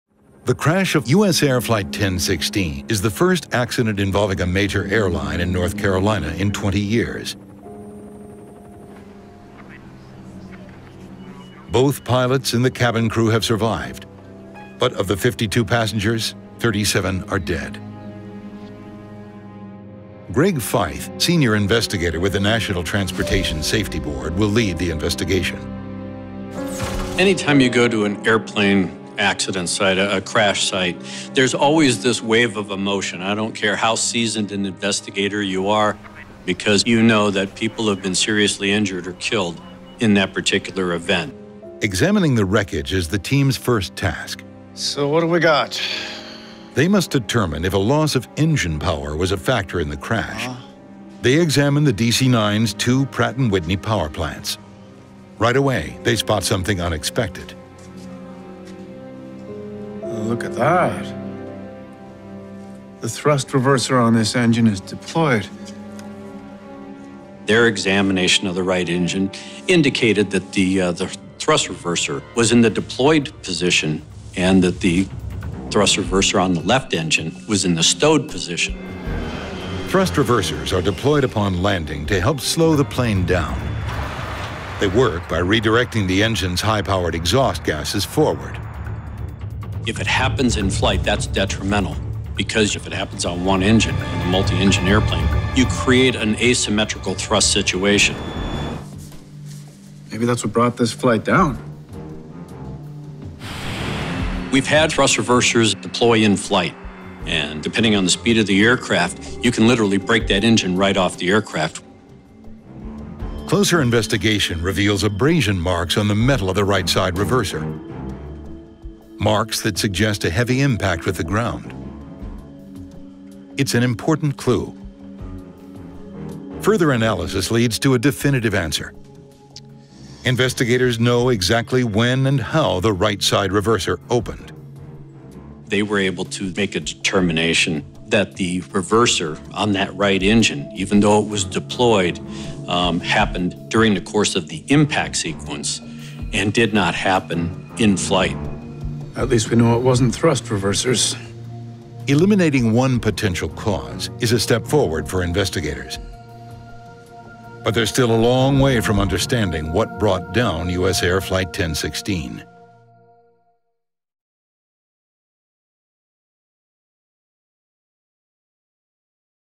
Did a Faulty Thrust Reverser Bring Down Flight 1016?
Thrust reversers are only supposed to deploy when a plane is landing. Did one of the thrust reversers on USAir Flight 1016 activate prematurely? And did that contribute to the crash on July 2, 1994?